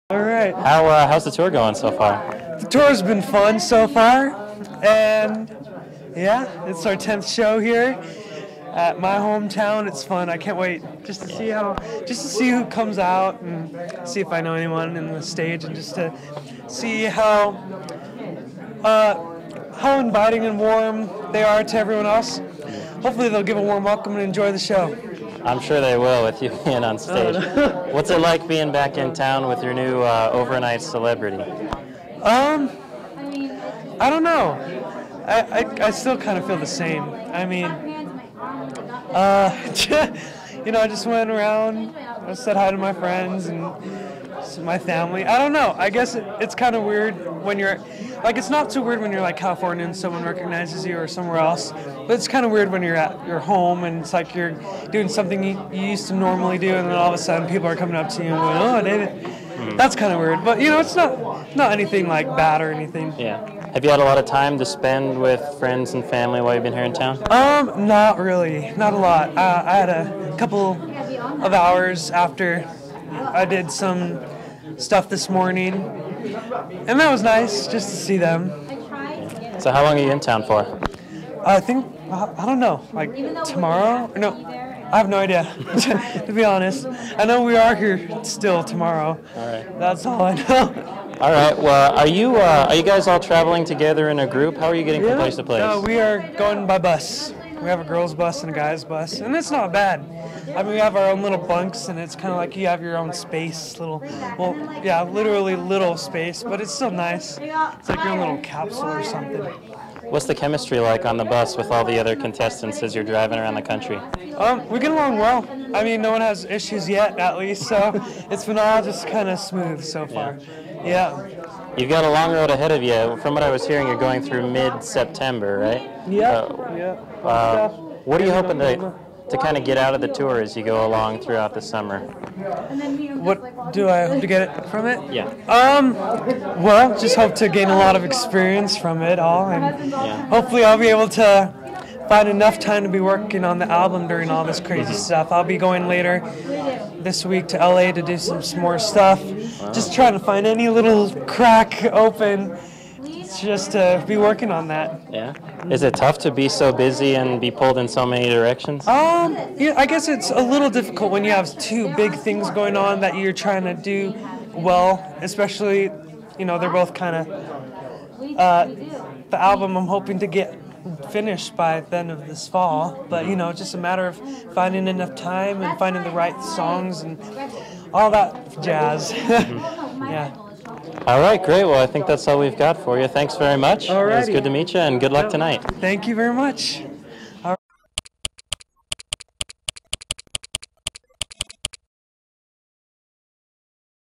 DHTV David Archuleta backstage
An interview with David Archuleta backstage before the 2008 American Idols Live tour in SLC.